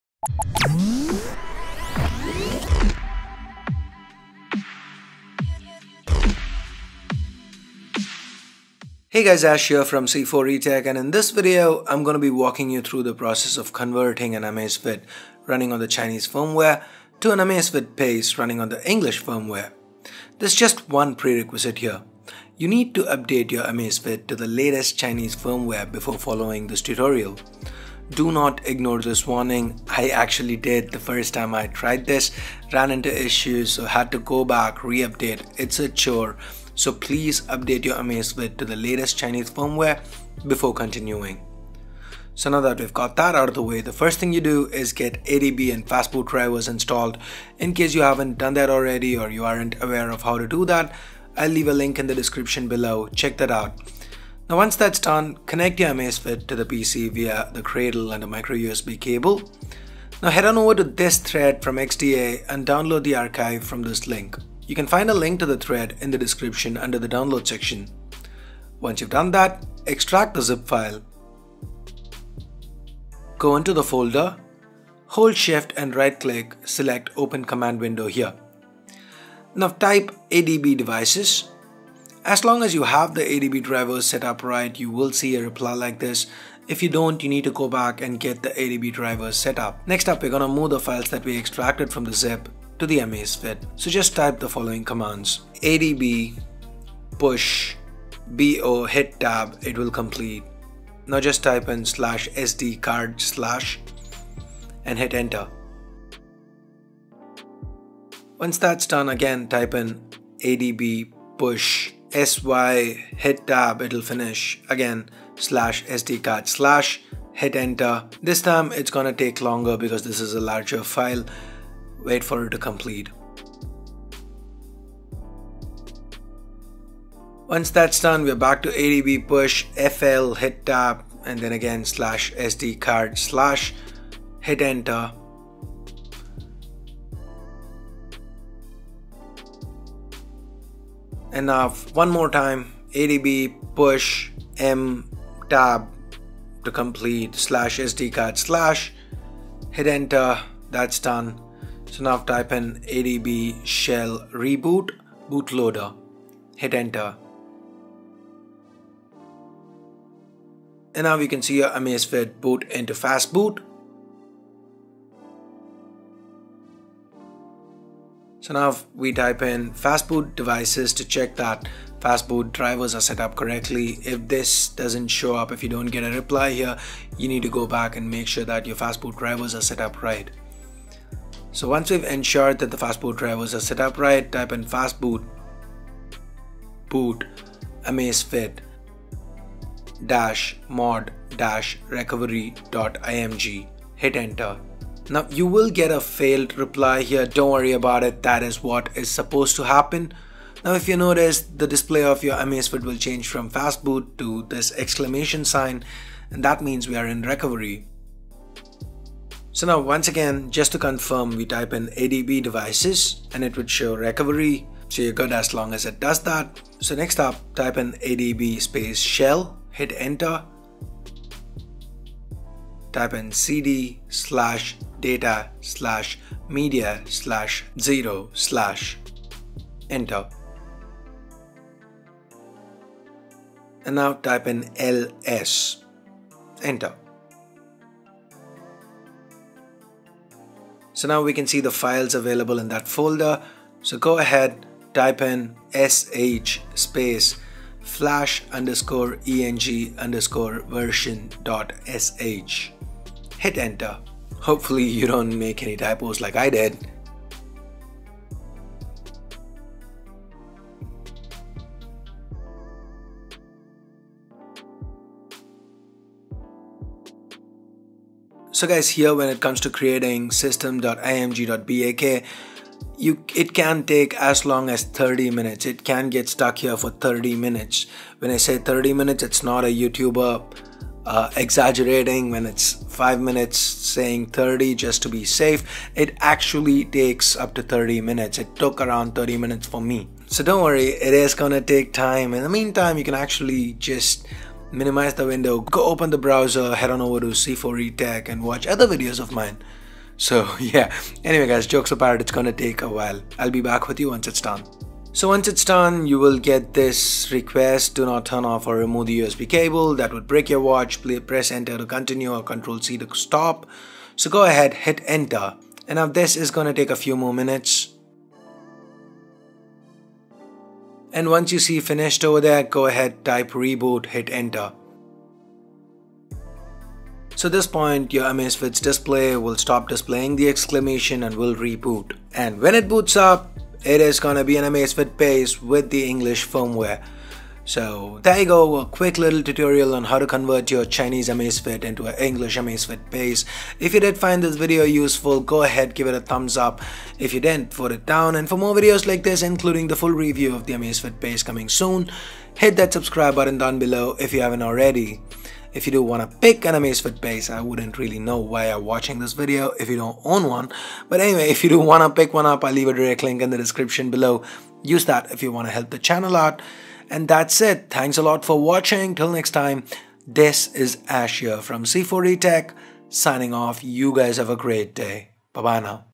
Amazfit - Chinese to English Conversion - A How to Tutorial!
A quick tutorial on how to convert the Xiaomi backed Huami's Amazfit from the Chinese firmware to English Firmware effectively converting it into an Amazfit Pace

Links

Where to buy the Amazfit